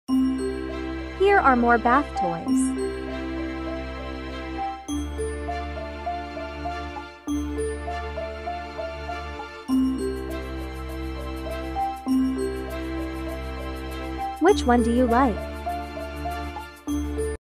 Kids Bath Toys |Keep them busy in the BATH 🐥 toytoyfunfun6 TikTok
Just fun little toys to keep the kids busy in the bath